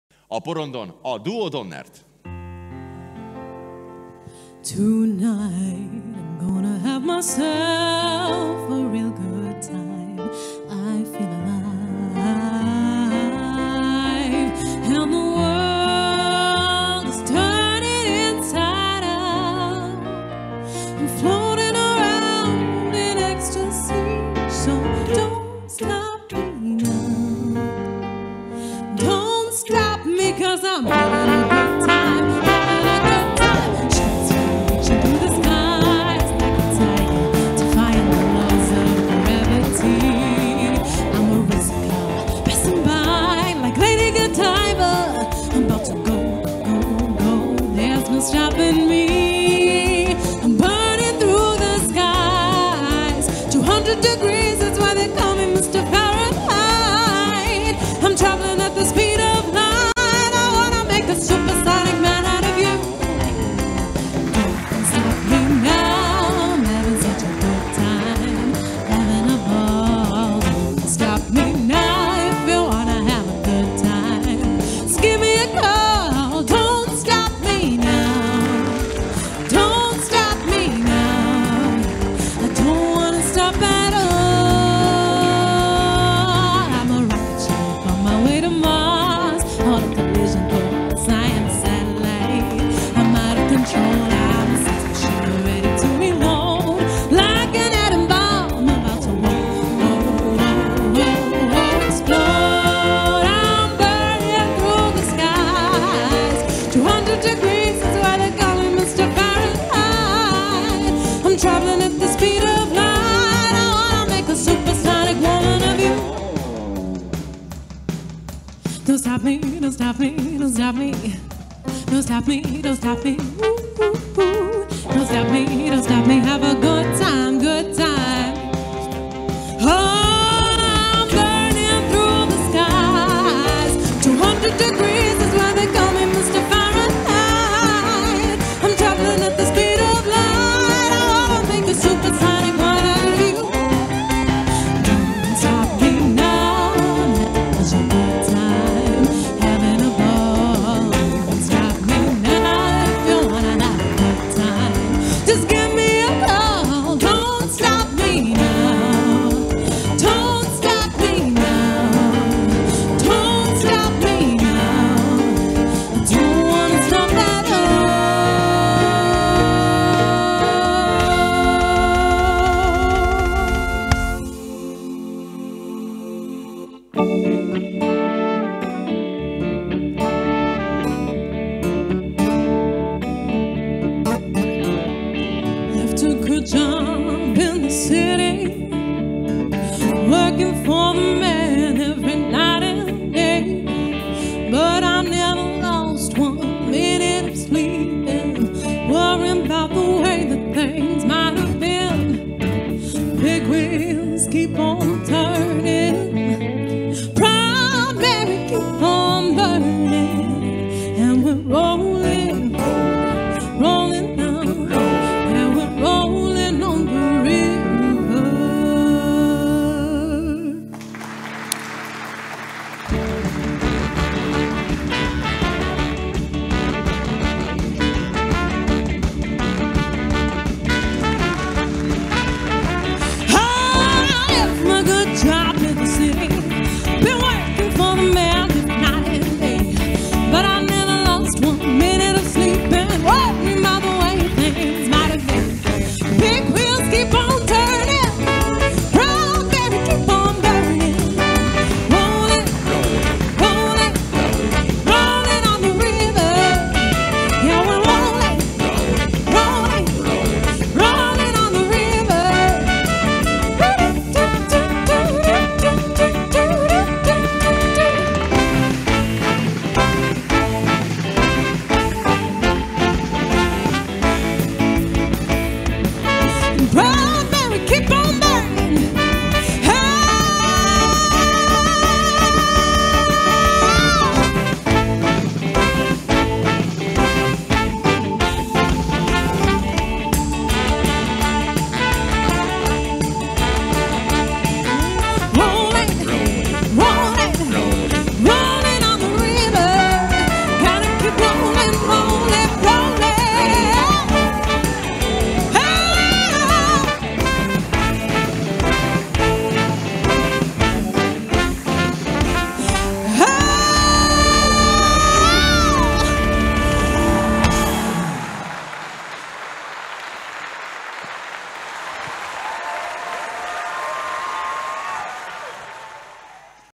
The New Duo Donnert Roller Skate Act
Ryan Donnert and Anna in the Capital Circus Budapest, performing the brand new Roller Skating Act.